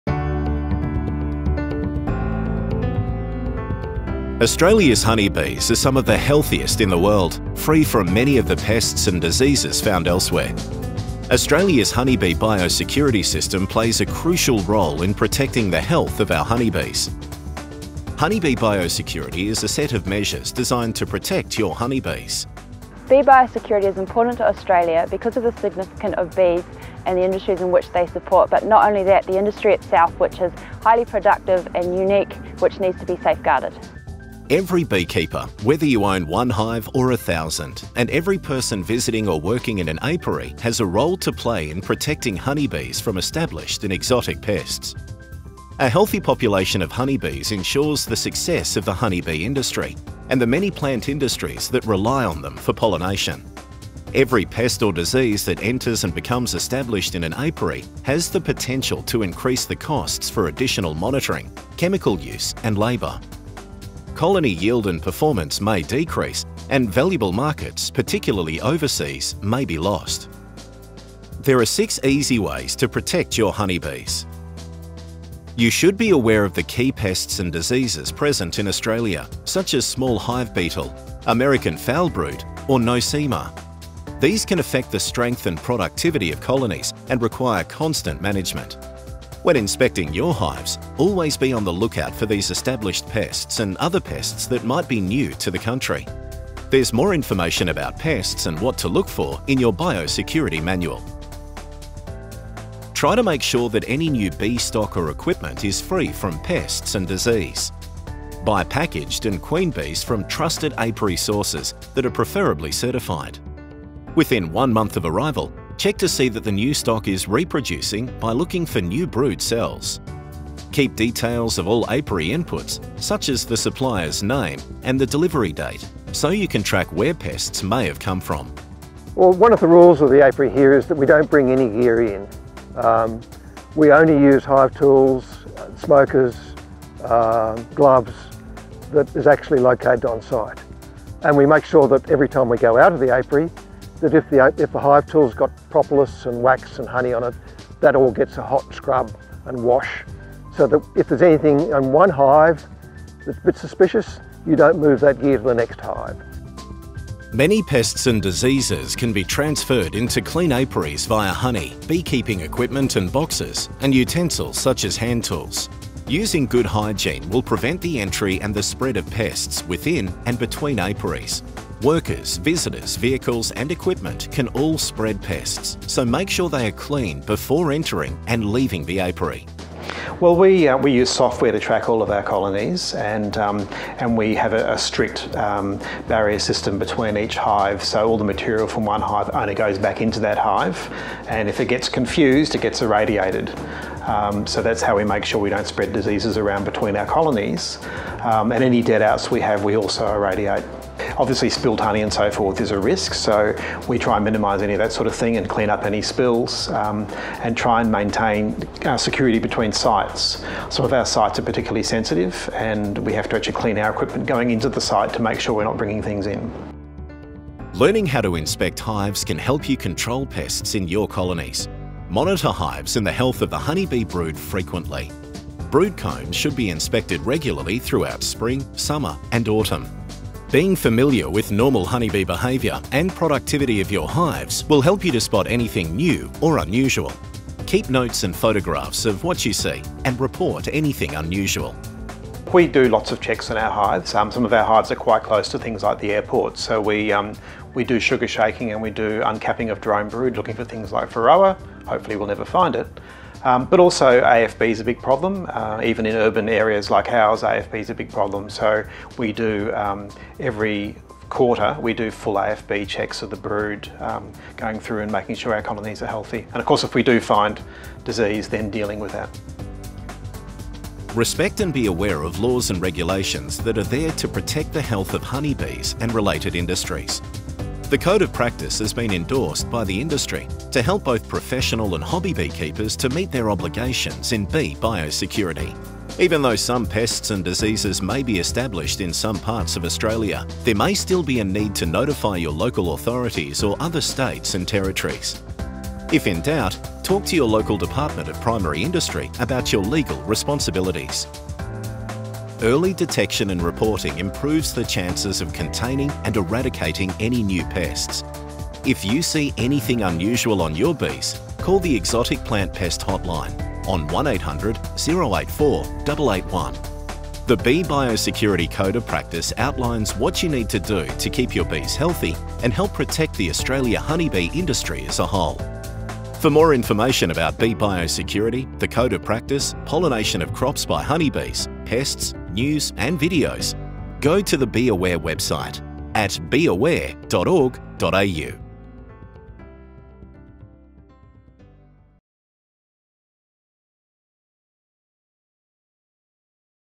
What is honey bee biosecurity?
BeeAware: Bee Biosecurity Video Series. Video 1. What is honey bee biosecurity?
Honey bee biosecurity is a set of six measures to assist beekeepers in knowing how to protect your honey bees from established and exotic pests. Early detection and reporting improves the chances of containing and eradicating any new pests.